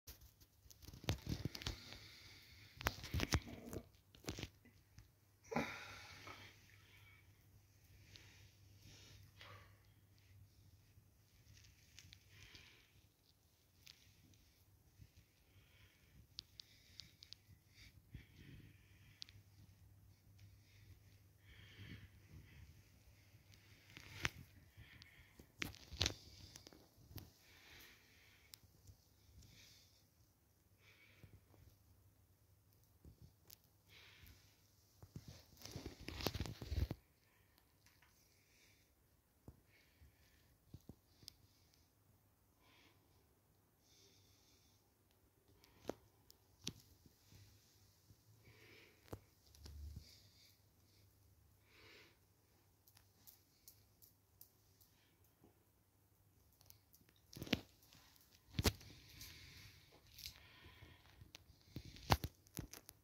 Crossdressing in red!!
Hello everyone. Welcome to another video.In this one a bright colour for you! I am wearing a red midi dress (or least  midi on me !)